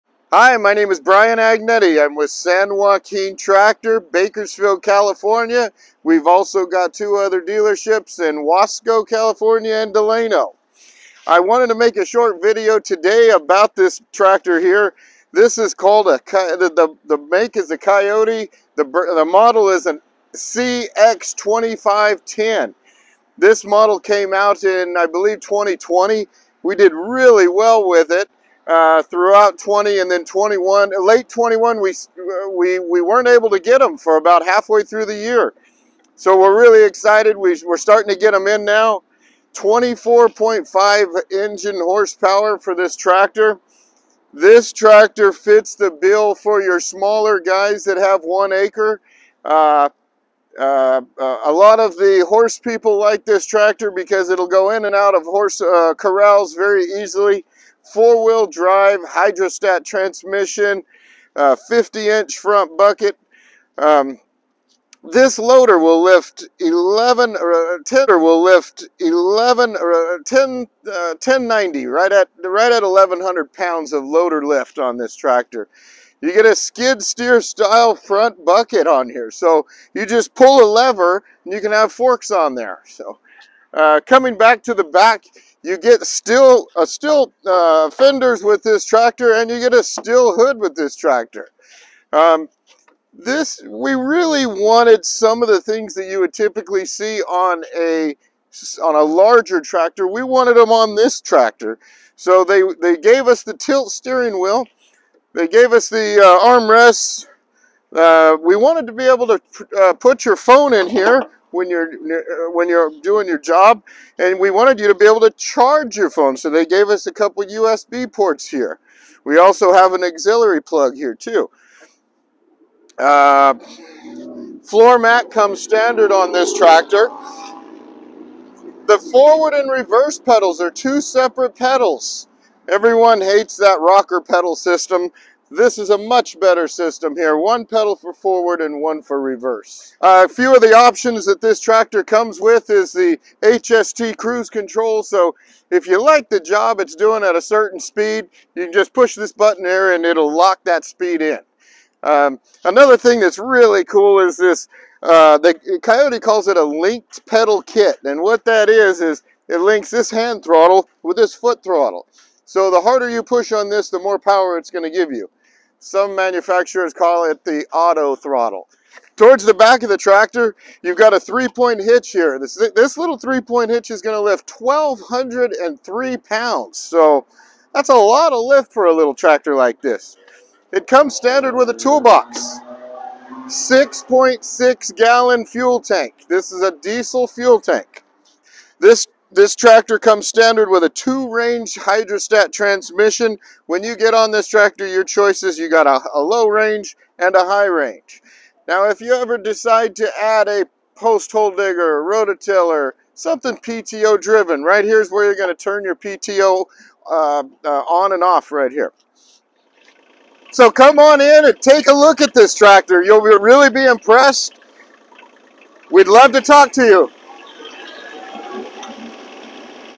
Kioti CX2510HB walk around. San Joaquin Tractor Bakersfield Ca
Engine Model         3A165LWH-U2
Engine Gross Power 24.5 hp (18.2 kW)
Rated Speed                 2,600 rpm
Aspiration                 Natural
Displacement         87 cu. in
Fuel Tank Capacity 6.6 gal. (25L)
Transmission
Transmission Type Hydrostatic
Main Gear                 Hydostatic
No. Travel Speeds Infinite/2R
Min. Travel Speed 0 mph
Max. Travel Speed 10.7 mph
Power Take-Off (PTO)
PTO Type Transmission Live
PTO Power                  18.8 hp
Rear PTO Speeds          540 rpm
Mid PTO Speeds          2,000 rpm (Optional)
Brakes and Clutch
Brake                         Wet Disc Type
Clutch                         Dry Single
Dimensions w/ Ag Tire
Length w/ 3-point 109.08 in.
Width 49.4 in. (1,256 mm)
Height from Top of ROPS 82.8 in.
Front Tread Ag Tires - Min 38.1 in.
Rear Tread Ag Tires - Min 37.4 in.
Min. Turning Radius w/Brake 8.3 ft.
Hydraulics
Total Pump Flow            11.3 gpm
3-Point Hitch
Standard Category Category 1
Lift Capacity, 24" aft of hitch 1,203 lbs